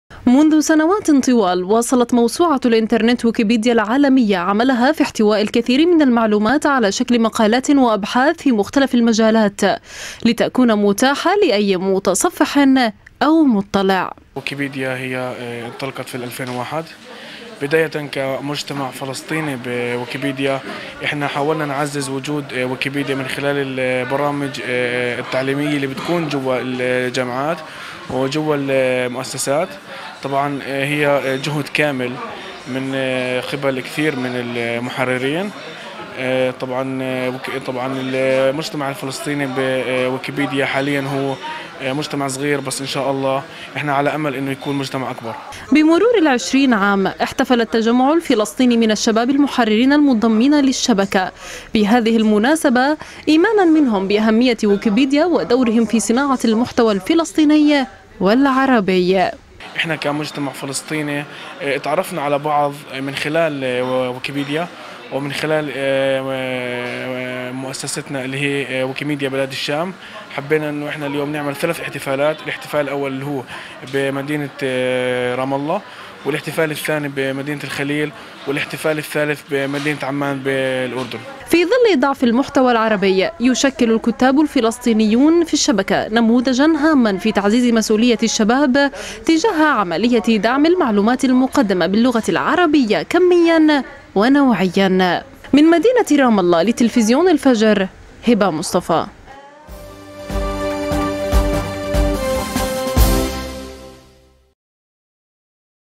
التجمع الفلسطيني يحتفل بمرور 20 عاماً على ويكيبيديا
تلفزيون الفجر | التجمع الفلسطيني يحتفل بمرور 20 عاماً على ويكيبيديا
تقرير: هبة مصطفى ، تصوير ومونتاج: براء عمر